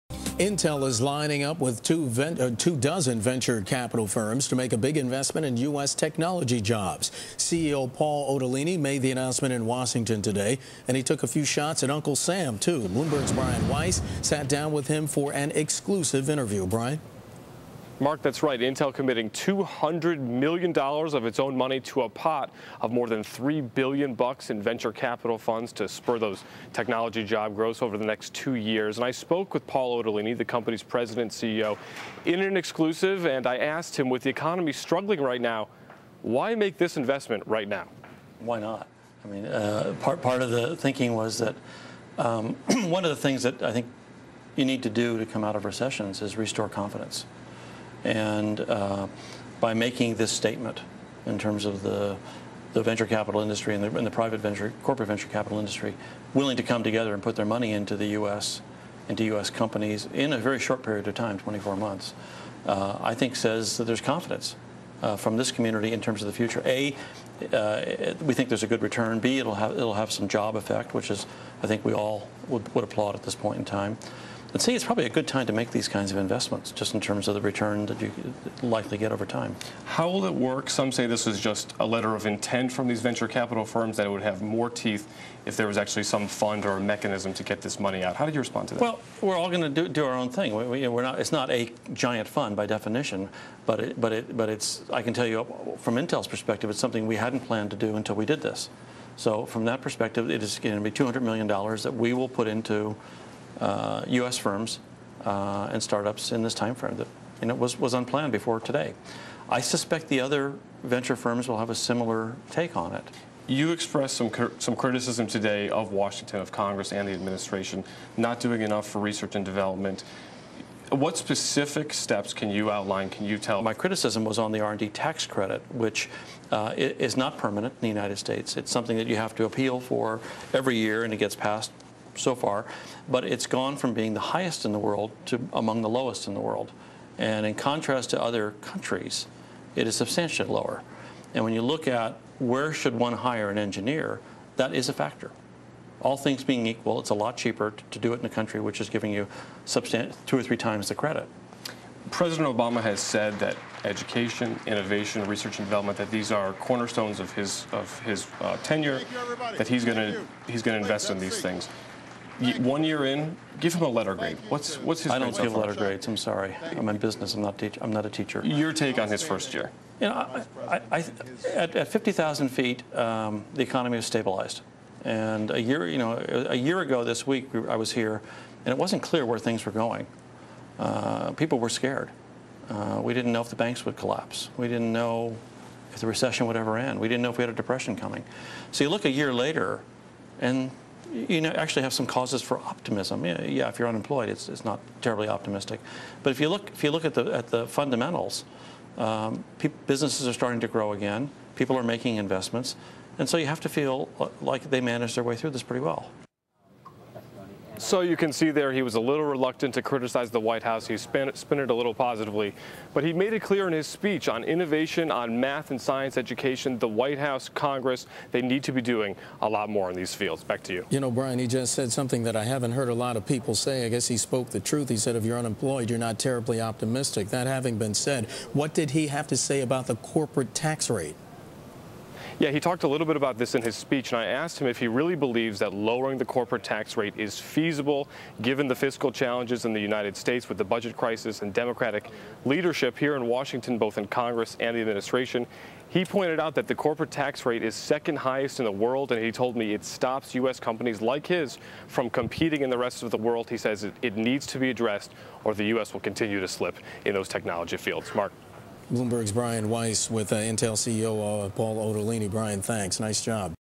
Otellini Says Now Is a Good Time to Invest in Technology: Video
Feb. 23 (Bloomberg) -- Paul Otellini, chief executive officer of Intel Corp., talks with Bloomberg's Brian Weiss about the company's plan to invest $200 million in U.S. technology companies over the next two years to spur domestic job growth.
     The investment is part of a $3.5 billion effort by a group of 24 venture-capital firms focusing on information technology, biotechnology and so-called clean technology. (Source: Bloomberg)